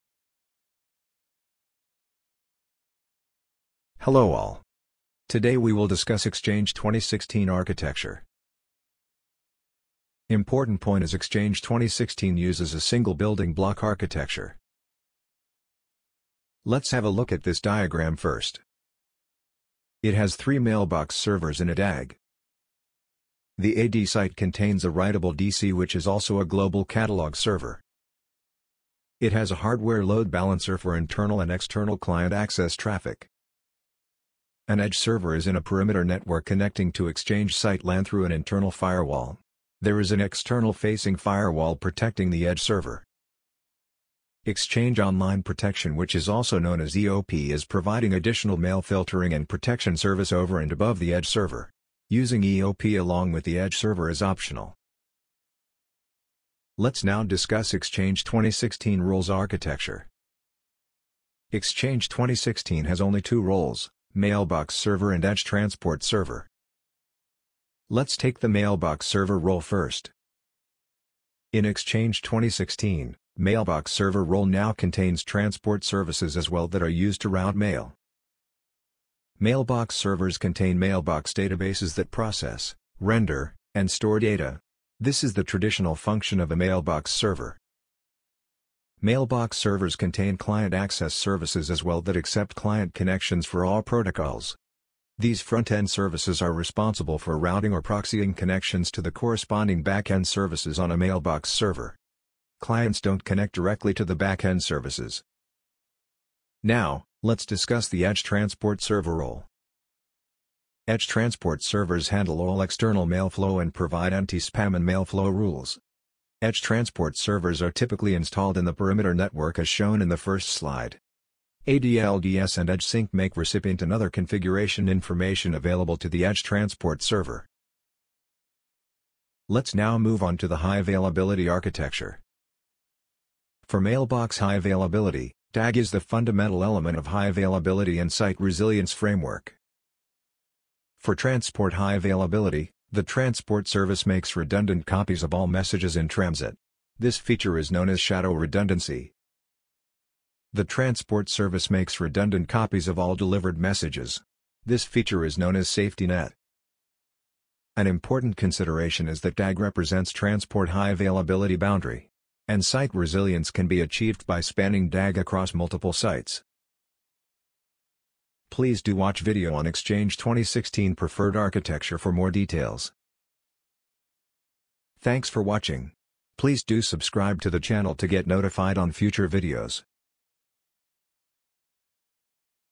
Exchange 2016 Architecture - In Short Technical Details
Technical details on Exchange 2016 server role architecture and high availability architecture in 3 minutes.